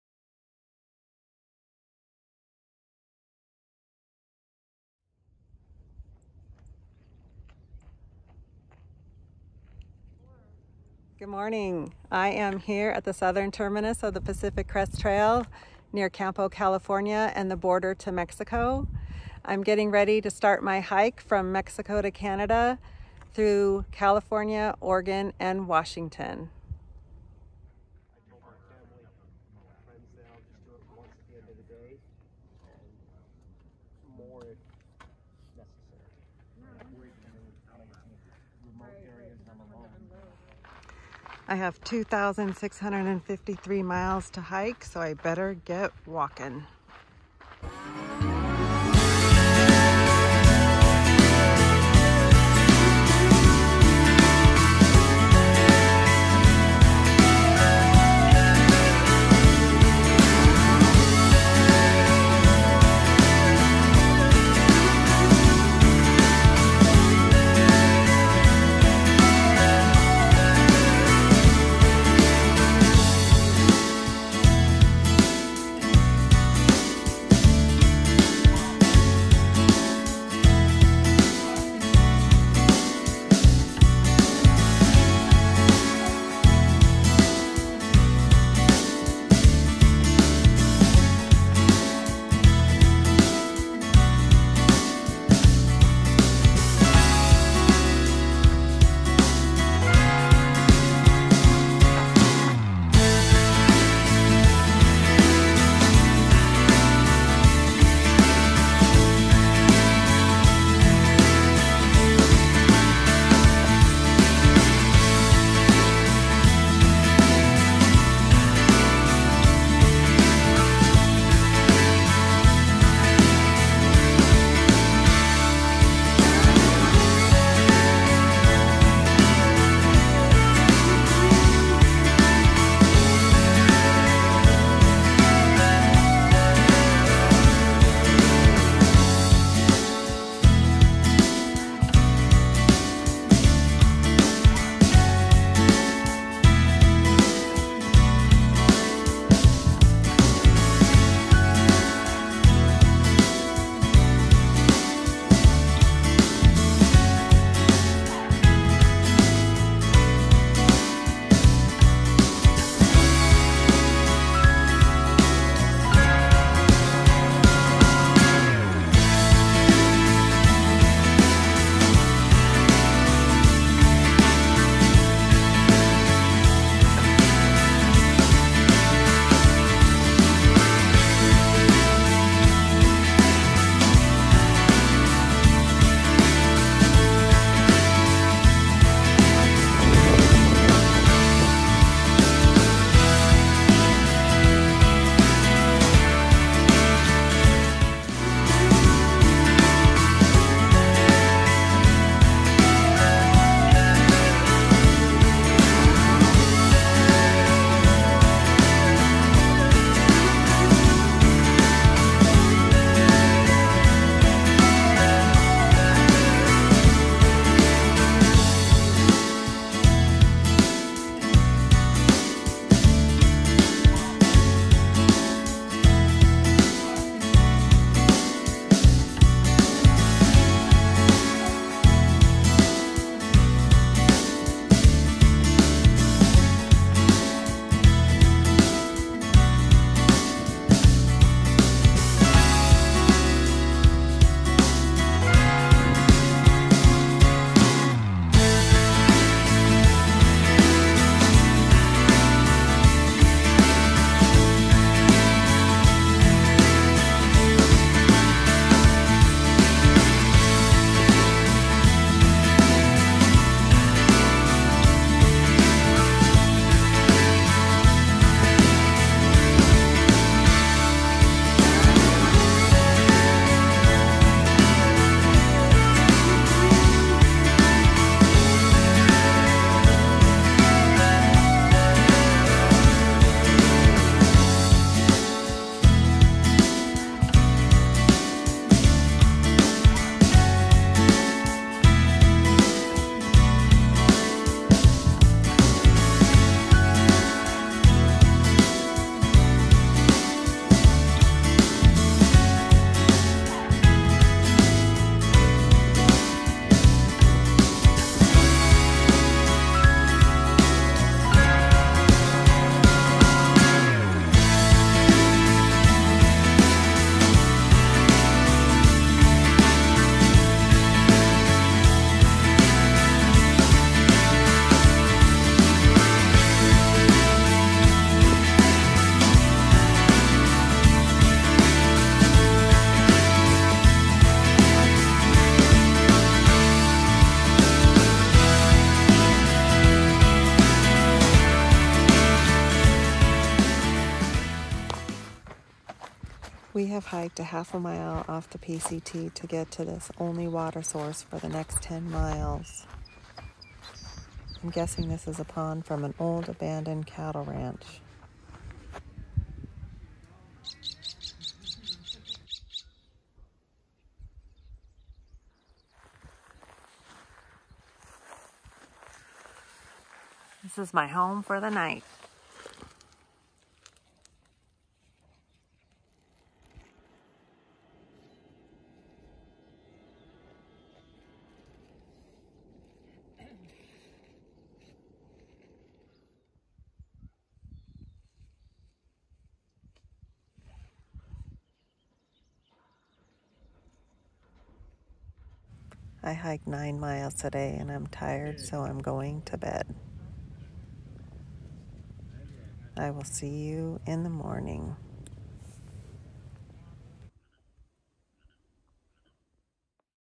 PCT 2021 - Day 1
April 15, 2021.  Mile 0 to mile 9. Hiked .5 off trail to get water from a pond at an old abandoned farm and camped there for the night. It was the only water source for the next 10 miles.